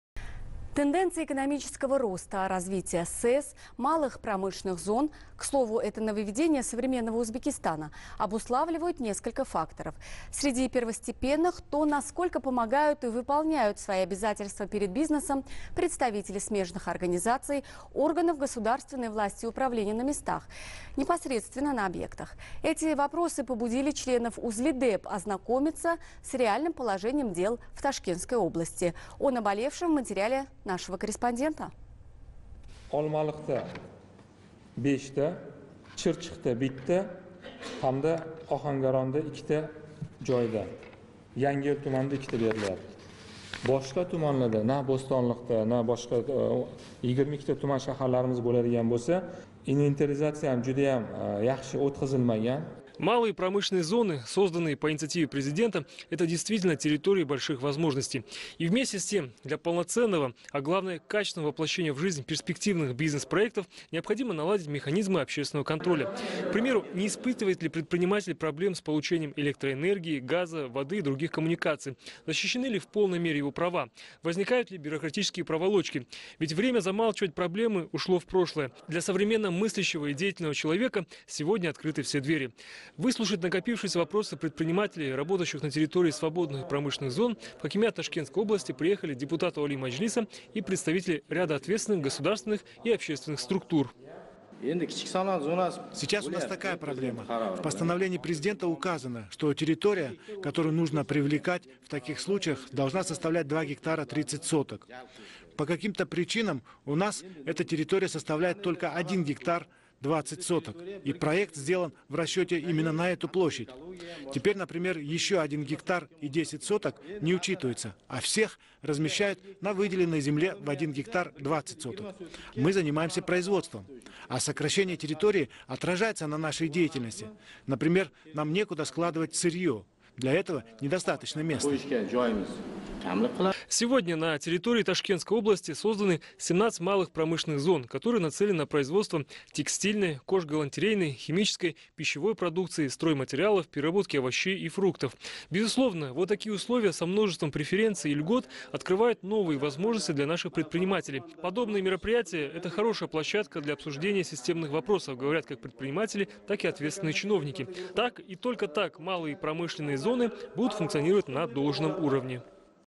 УзЛиДеП в Ташкентской области
Тенденции экономического роста, развития СЭЗ, малых промышленных зон – к слову, нововведение  современного Узбекистана, обуславливают несколько факторов. Среди первостепенных то, насколько помогают и выполняют свои обязательства перед бизнесом представители смежных организаций, органов гос.власти и управления на местах. Непосредственно на объектах. Эти вопросы побудил членов УзЛиДеп ознакомится с реальным положением дел в Ташкентской области. О наболевшем в материале нашего корреспондента.